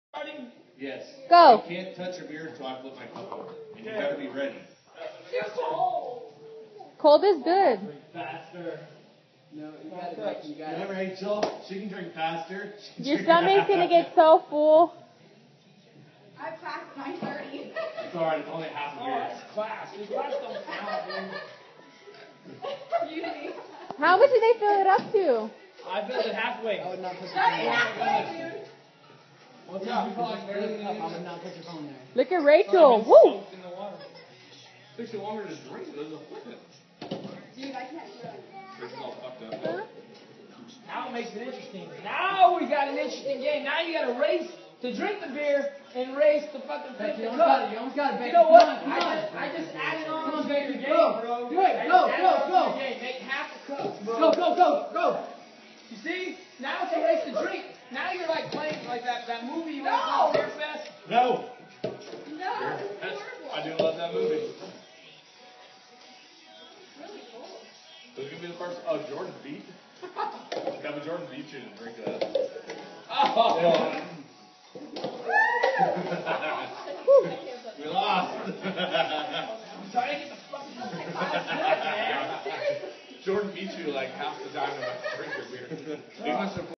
More flip cup
Playing flip cup, the new guy to play filled the cups up way to high specially for the girls...